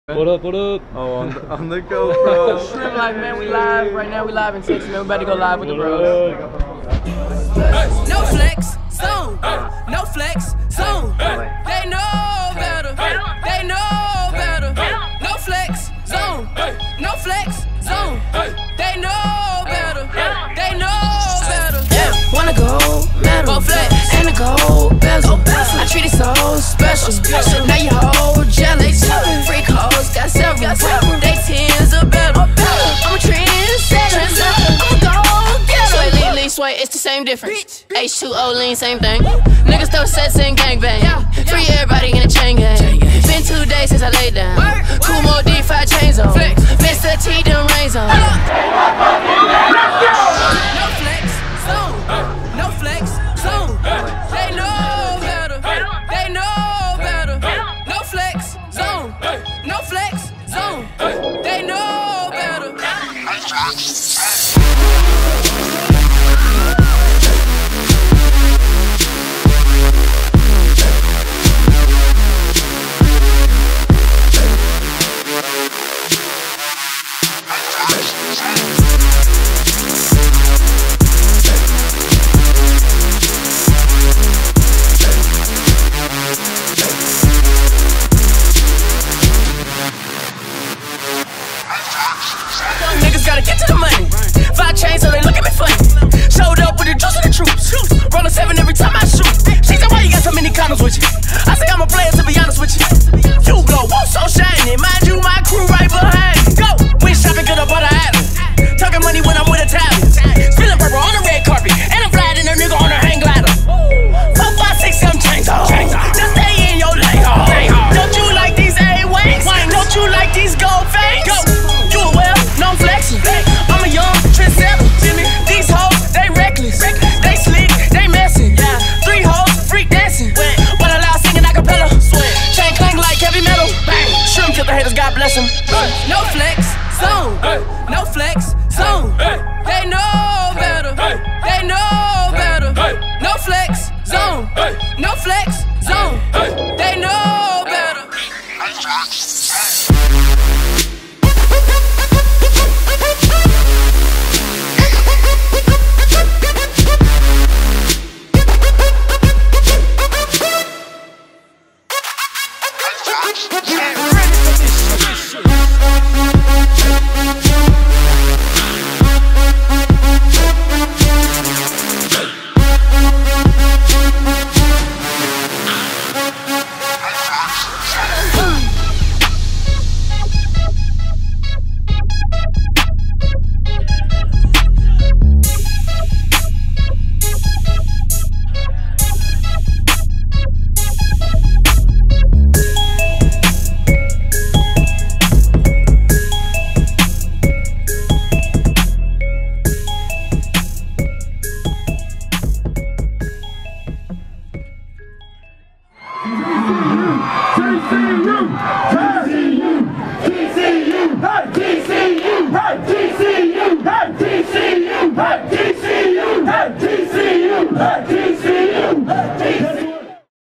TCU DELTA TAU DELTA RAE SREMMURD
TCU Delta Tau Delta Mardi Gras party 2015
Rush Delt!

Sigma Kappa & Gamma Phi Beta

Remix by SubSecond

Shout out to Swae Lee and Slim Jimmy!